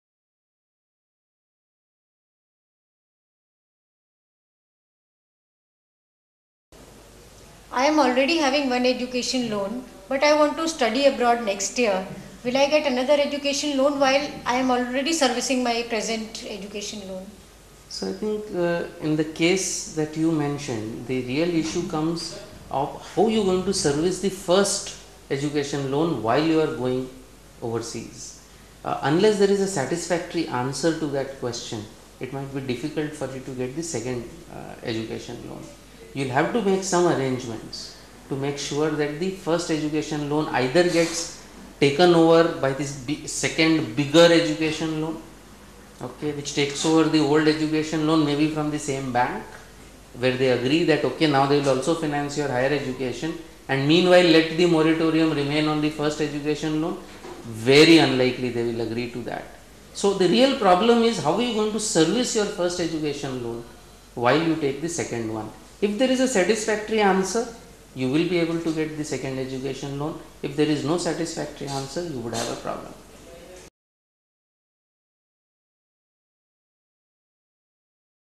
Have existing educational loan . Can I get another education loan for study abroad?
Harsh Roongta, CEO of advising people on Education Loan query viz.,Have existing educational loan . Can I get another education loan for study abroad?